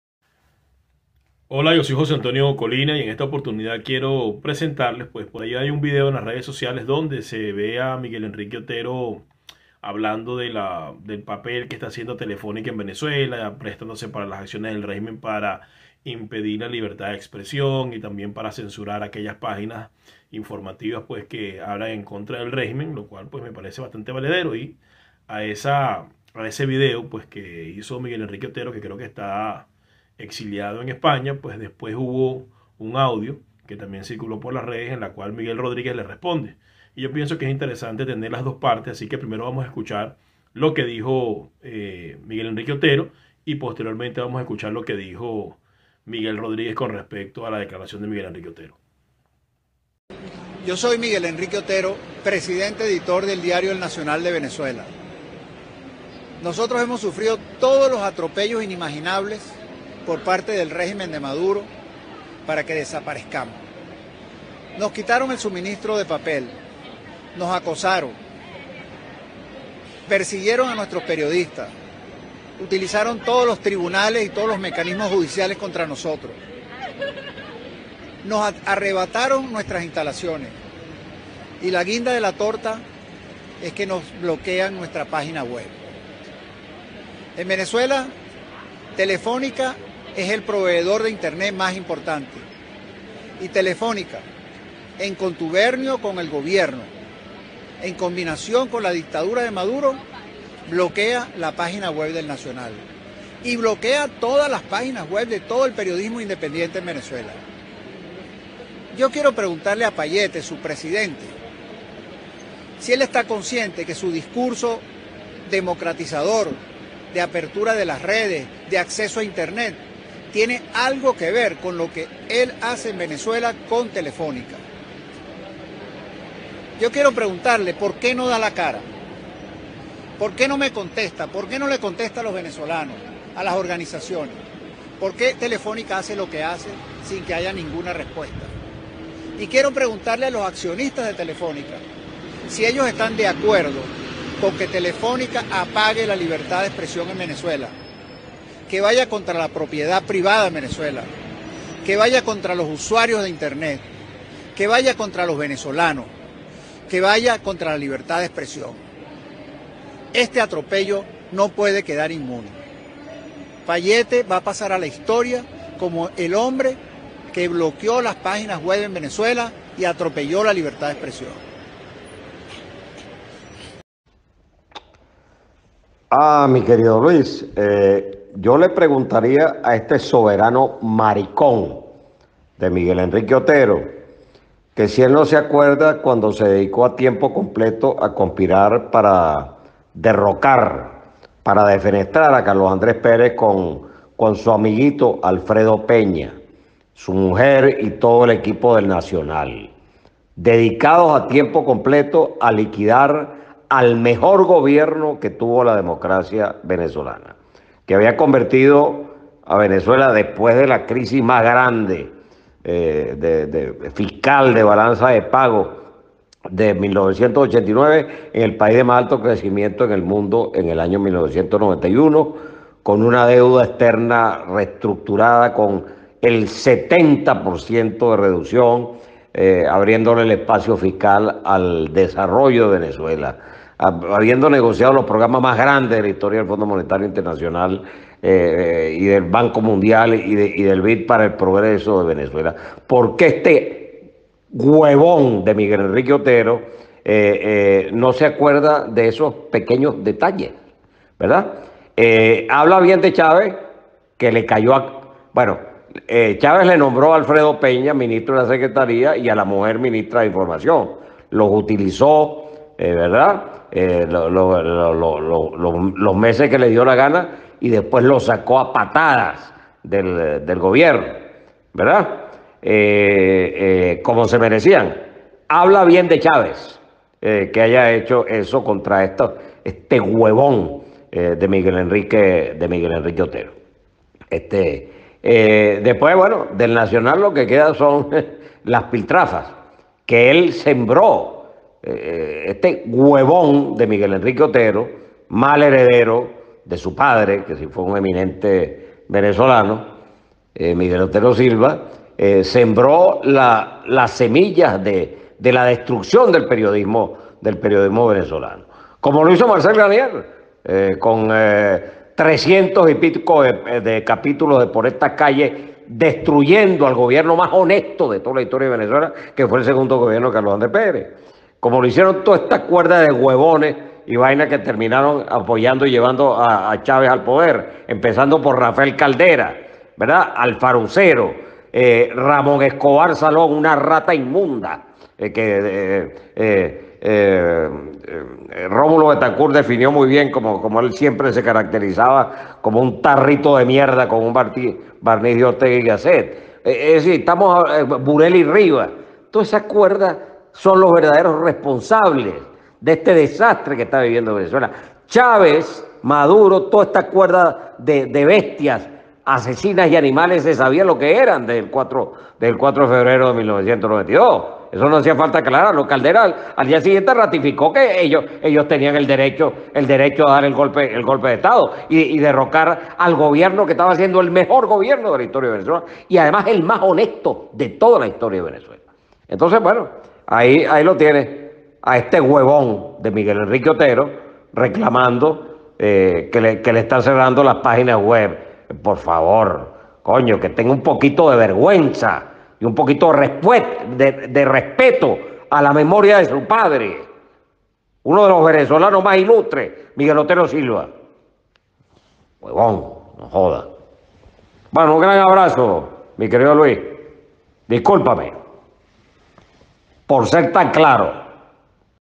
Miguel Rodríguez le responde a Miguel Henrique Otero
En un audio Miguel Rodríguez , ex ministro de Cordiplan  le responde a Miguel Henrique Otero dueño del Nacional sobre un vídeo donde, Miguel Henrique crítica las acciones de la empresa Telefónica en Venezuela para ayudar al régimen en la censura a los medios de información digitales.